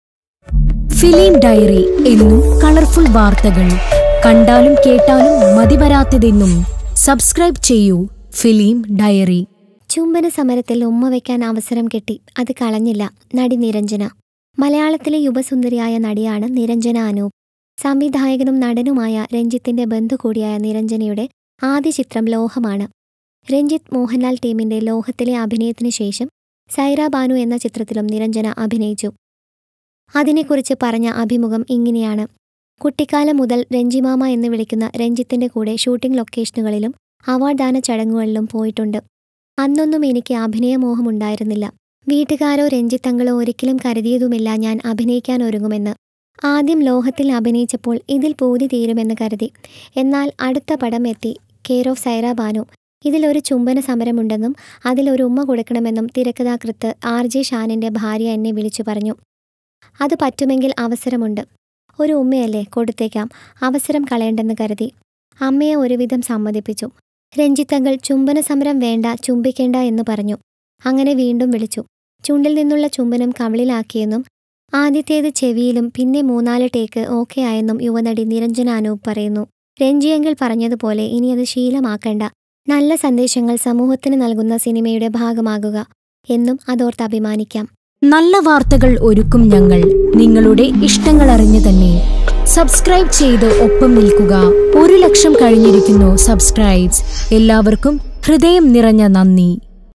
നടി നിരഞ്ജന ചുംബന സമരത്തിൽ പങ്കെടുത്തു,ഉമ്മ വെച്ചത് ക്യാമറ മാനേ
Watch Niranjana Anoop is a star who has gained a place in the minds of Malayalees. Then I said there was a scene of a kiss strike in it. Then I asked Shawn Chetna if he could keep Umm as a direct. Chettan said he wanted to. Finally, I said, and put that kiss on the cheek. And so that shot arrived. This is the first time a scene like this is being done. The first kiss was given in the ear. Then twelve times that kissing take was perfection